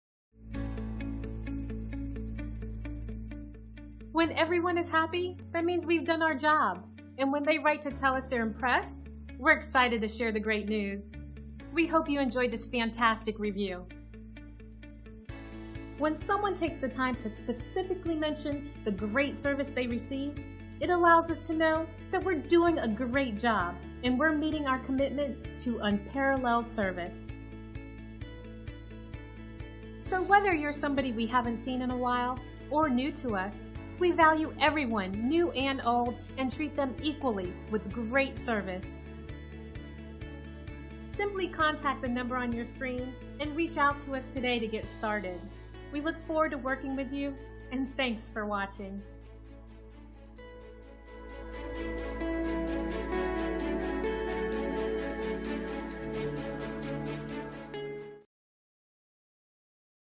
Treemendous Tree Care Inc. New Castle Excellent5 Star Review by Thomas L.
New Review

Courteous, professional, talked over various options without nickel and dime-ing, on time, cleaned-up the yard - small business man who values giving personal serviced. And priced lower than larger operations.

Treemendous Tree Care Inc.

New Castle  DE
19720